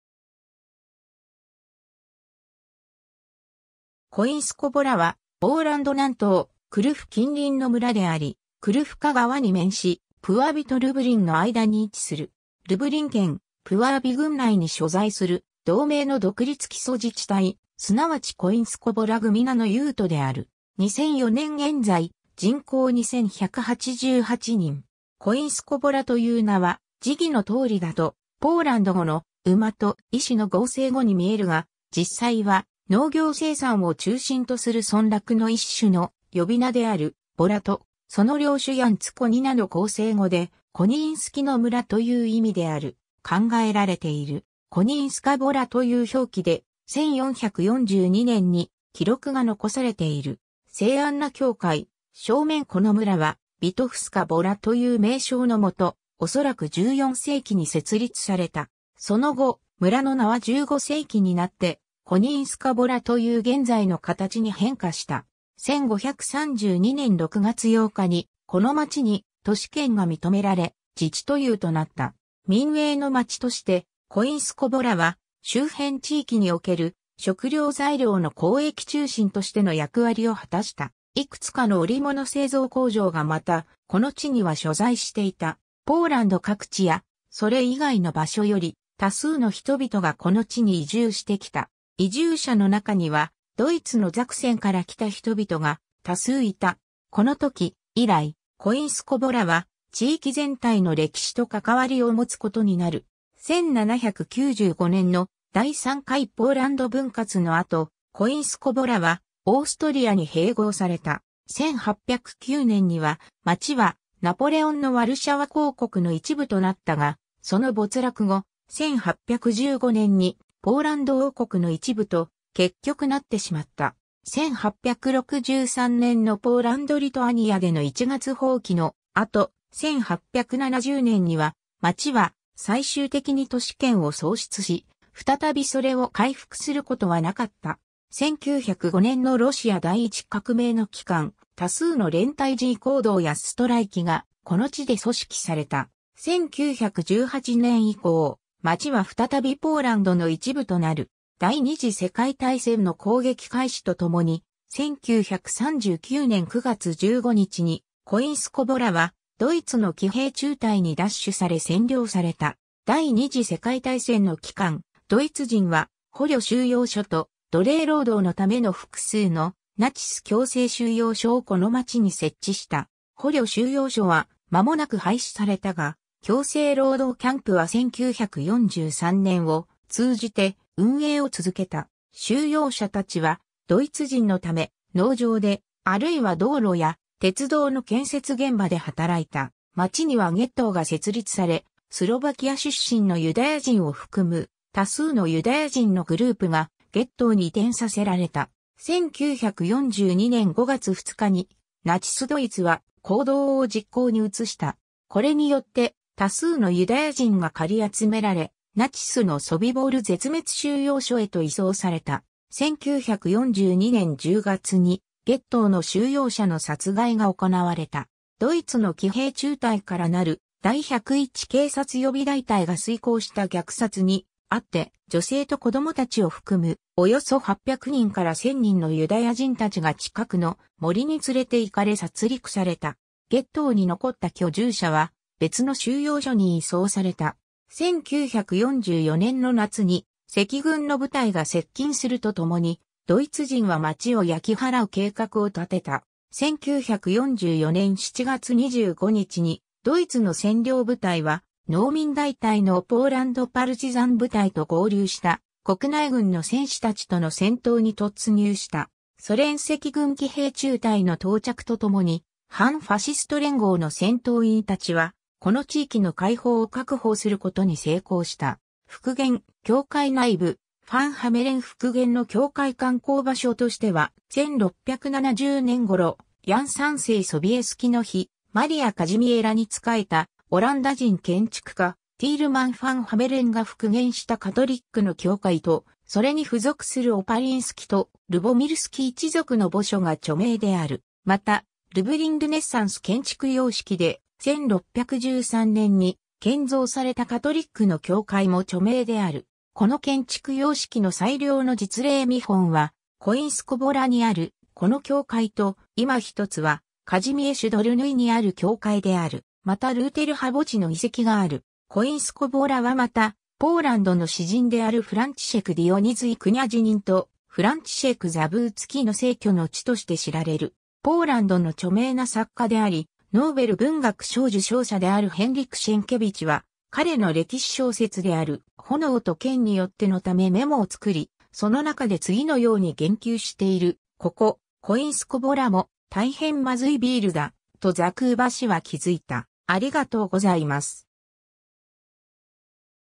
コィンスコヴォラ
コィンスコヴォラ（Końskowola）は、ポーランド南東、クルフ近隣の村であり、クルフカ川（EN）に面し、プワヴィとルブリンのあいだに位置する。
ルブリン県、プワーヴィ郡（ powiat puławski、EN）内に所在する、同名の独立基礎自治体、すなわちコィンスコヴォラ・ グミナ（群落）の邑都である。
2004年現在、人口2188人。
 コィンスコヴォラ（Końskowola）という名は、字義の通りだとポーランド語の「馬（コィン、Koń）」と「意志（ヴォラ、wola）」の合成語にみえるが、実際は、農業生産を中心とする村落の一種の呼び名である「ヴォラ（wola）」と、その領主ヤン・ツ・コニナ（ヤン・コニィンスキ、すなわちコニンのヤン）の合成語で、「コニィンスキの村」という意味である考えられている。
コニィンスカヴォラという表記で、1442年に記録が残されている。
 聖アンナ教会・正面 この村は、ヴィトフスカ・ヴォラという名称のもと、おそらく14世紀に設立された。
その後、村の名は15世紀になって、コニィンスカヴォラという現在の形に変化した。
 1532年6月8日に、この町に都市権が認められ、自治都邑となった。
民営の町として、コィンスコヴォラは周辺地域における食料材料の交易中心としての役割を果たした。
幾つかの織物製造工場がまた、この地には所在していた。
ポーランド各地や、それ以外の場所より、多数の人々がこの地に移住してきた。
移住者のなかには、ドイツのザクセンから来た人々が多数いた。
 このとき以来、コィンスコヴォラは地域全体の歴史と関わりを持つことになる。
1795年の第3回ポーランド分割の後、コィンスコヴォラはオーストリアに併合された。
1809年には、町はナポレオンのワルシャワ公国の一部となったが、その没落後、1815年にポーランド王国の一部と結局なってしまった。
1863年のポーランド・リトアニアでの一月蜂起の後、1870年には、町は最終的に都市権を喪失し、再びそれを回復することはなかった。
1905年のロシア第一革命の期間、多数の連帯示威行動やストライキが、この地で組織された。
1918年以降、町は再びポーランドの一部となる。
 第二次世界大戦の攻撃開始と共に、1939年9月15日に、コィンスコヴォラはドイツの騎兵中隊に奪取され占領された。
第二次世界大戦の期間、ドイツ人は捕虜収容所と奴隷労働のための複数の「ナチス強制収容所」をこの町に設置した。
捕虜収容所は間もなく廃止されたが、強制労働キャンプは1943年を通じて運営を続けた。
収容者たちは、ドイツ人のため、農場で、あるいは道路や鉄道の建設現場で働いた。
 町にはゲットーが設立され、スロヴァキア出身のユダヤ人を含む多数のユダヤ人のグループがゲットーに移転させられた。
1942年5月2日に、ナチス・ドイツは「行動」（アクツィオン、Aktion ）を実行に移した。
これによって、多数のユダヤ人が駆り集められ、ナチスのソビボル絶滅収容所へと移送された。
1942年10月に、ゲットーの収容者の殺害が行われた。
ドイツの騎兵中隊からなる「第101警察予備大隊」が遂行した虐殺にあって、女性と子供たちを含む、およそ 800人から 1000人のユダヤ人たちが近くの森に連れて行かれ殺戮された。
ゲットーに残った居住者は、別の収容所に移送された。
 1944年の夏に赤軍の部隊が接近すると共に、ドイツ人は町を焼き払う計画を立てた。
1944年7月25日に、ドイツの占領部隊は、「農民大隊」（バタリヨニ・フウォプスキェ、Bataliony Chłopskie、EN）のポーランド・パルチザン部隊と合流した国内軍（アールミヤ・クラヨーヴァ）の戦士たちとの戦闘に突入した。
ソ連赤軍騎兵中隊の到着と共に、反ファシスト連合の戦闘員たちは、この地域の解放を確保することに成功した。
 復元・教会内部 ファン・ハメレン復元の教会 観光場所としては、1670年頃、ヤン3世ソビエスキの妃マリア・カジミェラに仕えたオランダ人建築家ティールマン・ファン・ハメレン（Tielman van Gameren、EN）が復元したカトリックの教会と、それに付属するオパリィンスキとルボミルスキ一族の墓所が著名である。
また「ルブリン・ルネッサンス」建築様式で、1613年に建造されたカトリックの教会も著名である。
この建築様式の最良の実例見本は、コィンスコヴォラにあるこの教会と、いま一つは、カジミエシュ・ドルヌィ（EN）にある教会である。
またル...